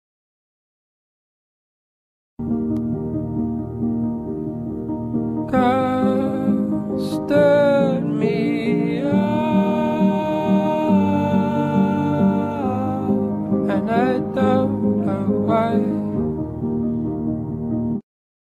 god...stood..me up~//Meme//Dead mirabel au//ORIGINAL MEME??//
characters used:
Mirabel madrigal (Her corpse/Dead au)
Bruno Madrigal
Julieta Madrigal
Abuela Madrigal (Alma)

apps used:
Capcut
Ibis paint x
Gacha Cute

info:
In this au casita didnt have enough magic to save mirabel from the falling tower. The pile of bricks hit her head so hard that it almost looked like a crushed watermelon julieta tried to feed her food for her too feel better
and bruno wished he saw it coming andabuela regretting every bad thing she said abt mirabel.They never moved on

Date:
1/25/22